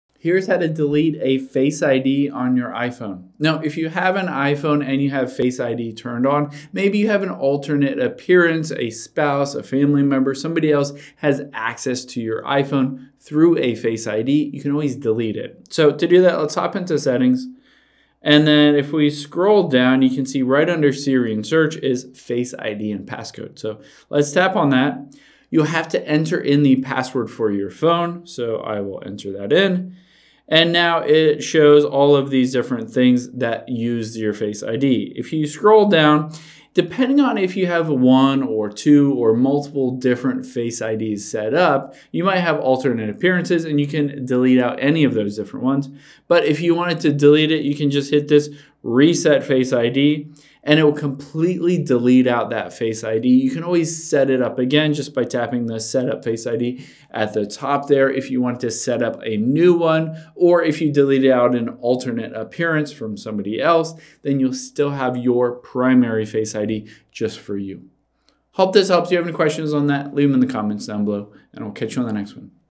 How To Delete Face ID On iPhone
Let's delete any of your Face IDs either for you or an alternate appearance on your iPhone.